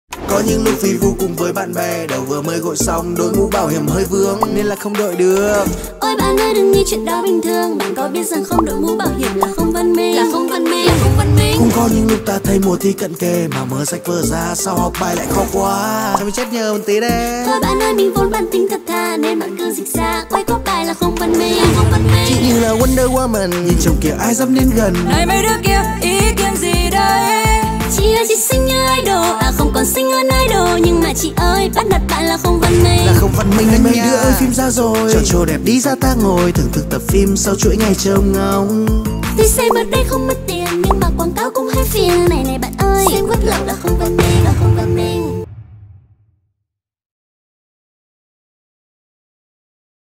Xem web lậu là không văn minh
Nhân ngày Sở hữu Trí tuệ thế giới 26/04/2021, hãy cùng Truyền hình K+ nói "Tôi không xem web lậu"

👉🏻 Đừng quên tham gia cuộc thi CÙNG NÓI “TÔI KHÔNG XEM WEB LẬU” để nhận được nhiều giải thưởng tiền mặt hấp dẫn tại